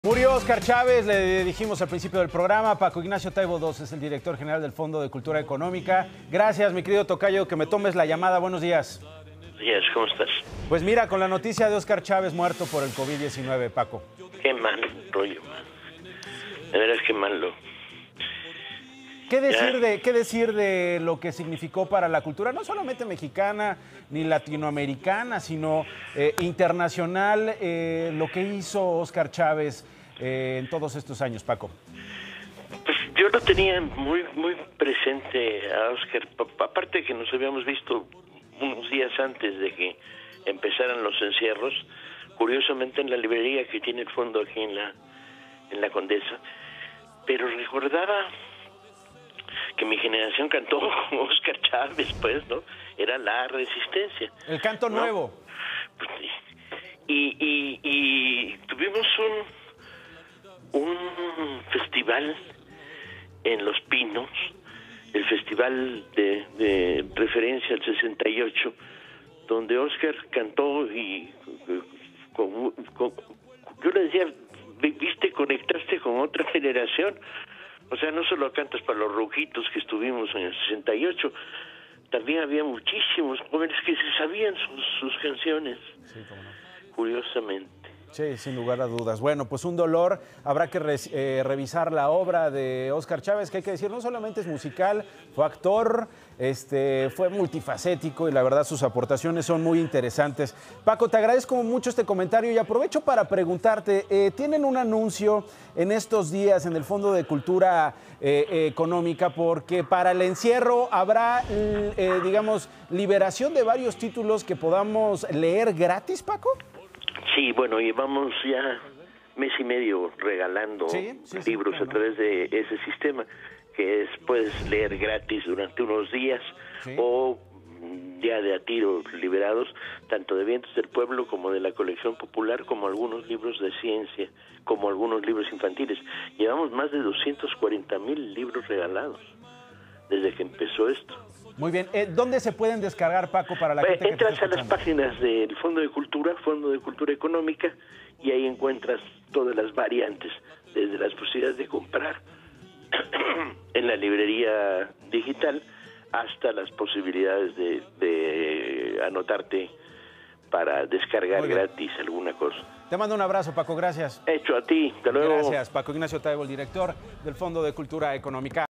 Paco Ignacio Taibo II habla del legado de Óscar Chávez | De Pisa y Corre
Resalta el director general del Fondo de Cultura Económica, Paco Ignacio Taibo II, la aportación cultural de Óscar Chávez tras su fallecimiento por COVID19.